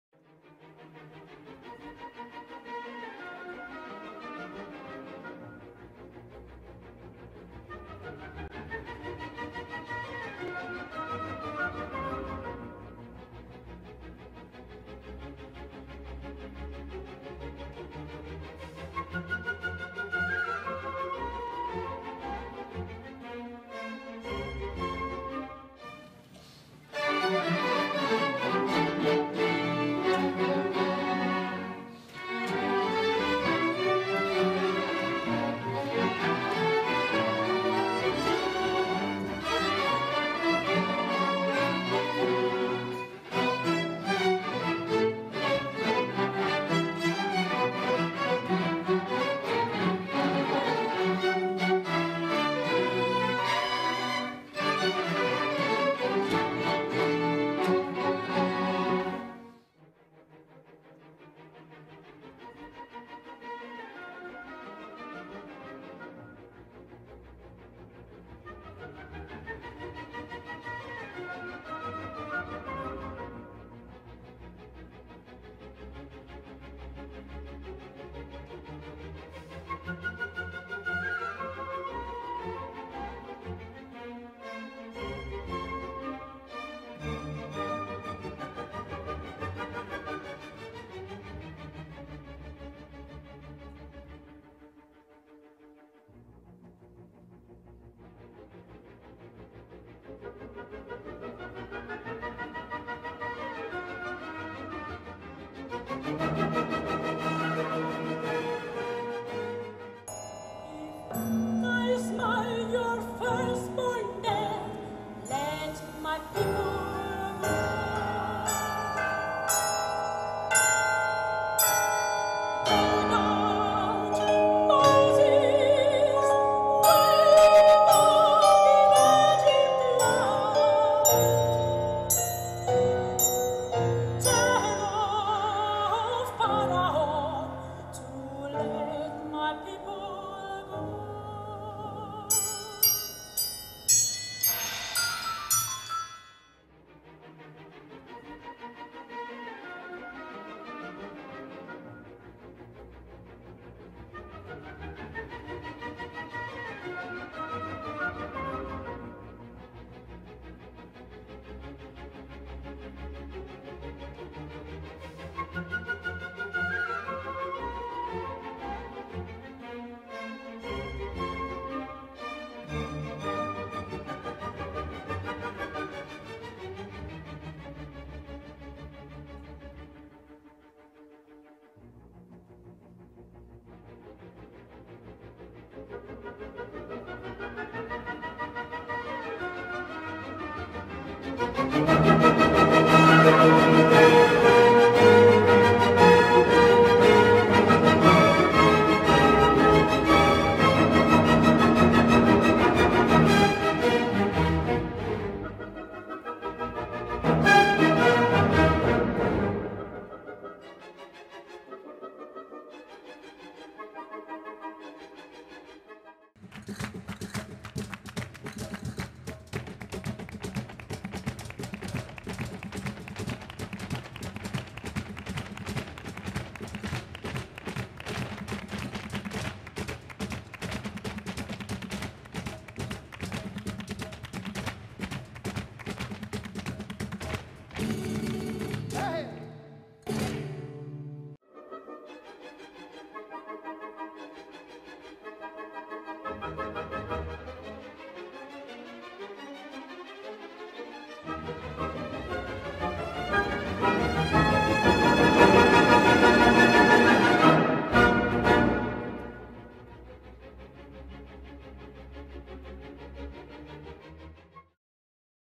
VIDEO PROMOCIONAL FIS 11-17 AGOSTO 2014
Video promocional de las actuaciones de la primera semana del Festival Internacional de Santander. Del 11 al 17 de agosto de 2014.